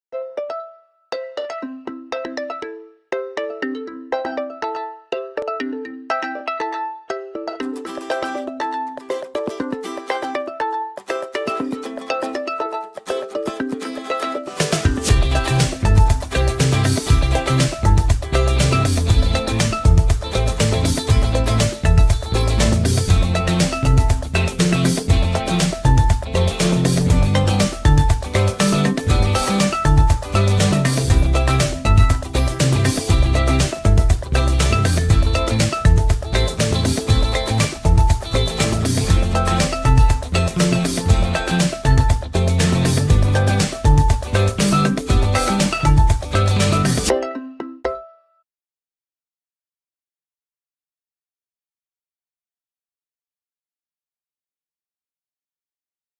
Музыкальная открытка, посвящённая Дню знаний
Эту небольшую музыкальную открытку со своими стихами я посвятил одному из светлых праздников не только для первоклассников, но и для тех, кто уже давно закончил школу, а сейчас помогает проходить этот путь своим детям и внукам.